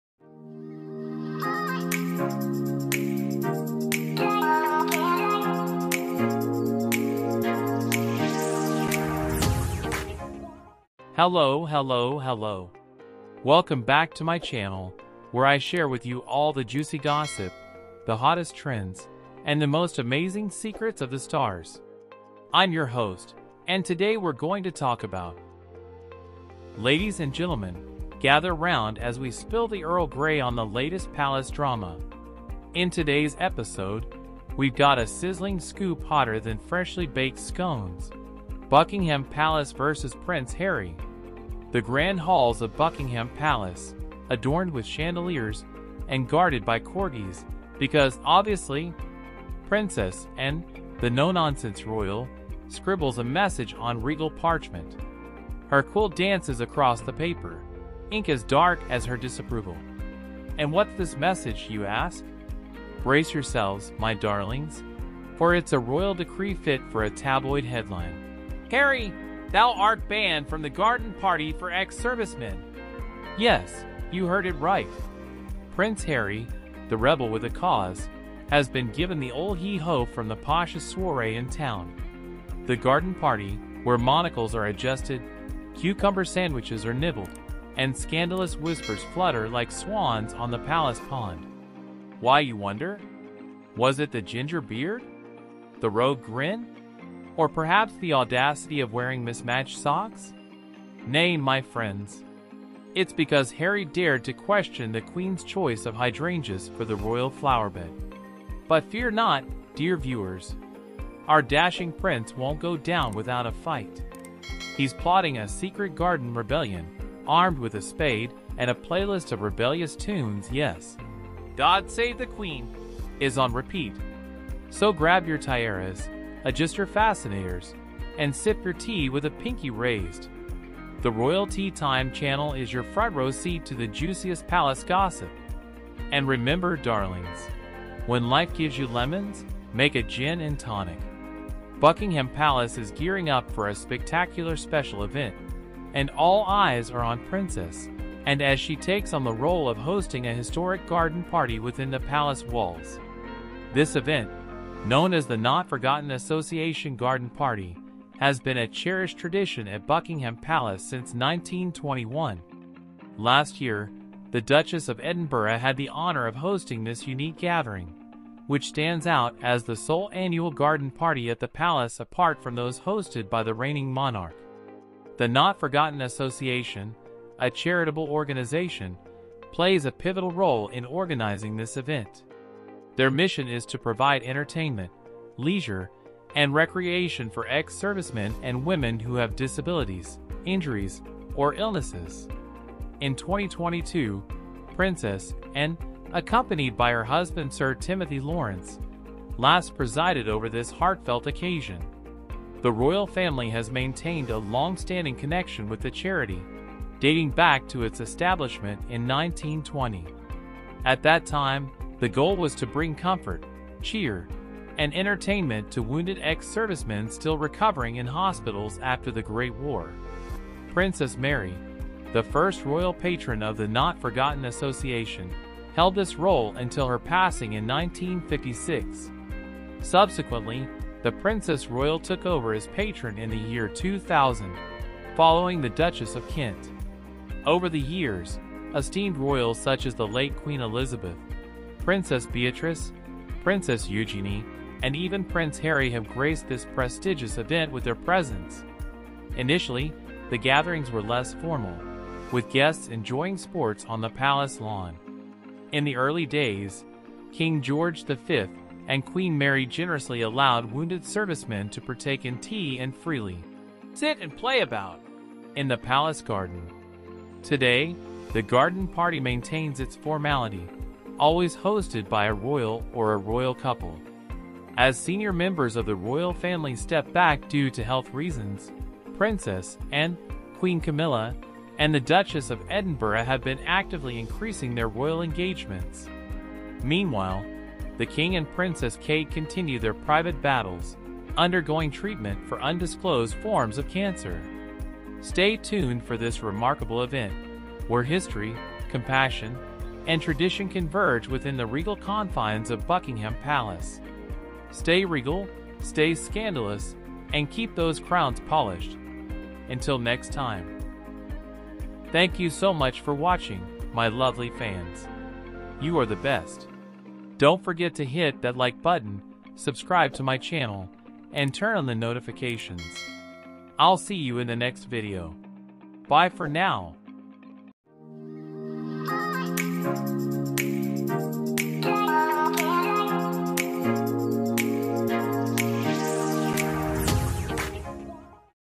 Royal Rift: Princess Anne’s Message to Harry Sparks Ban from Veterans’ Gala (Got Harry Banned)
Ladies and gentlemen, gather 'round as we spill the Earl Grey on the latest palace drama!
In today's episode, we've got a sizzling scoop hotter than freshly baked scones: Buckingham Palace vs. Prince Harry!